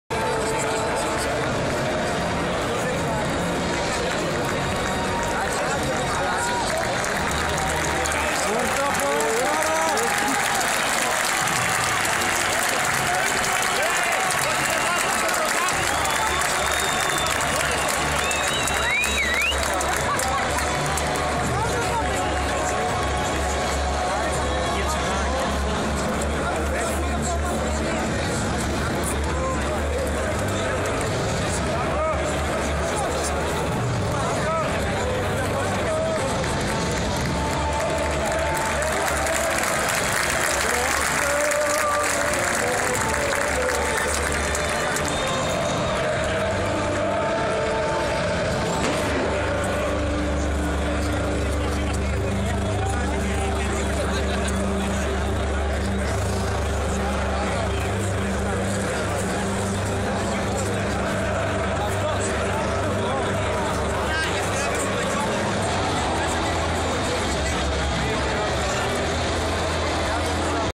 ΠΑΟΚ-Απόλλων Σμύρνης 30/01/2014 Τούμπα
Η είσοδος των ομάδων.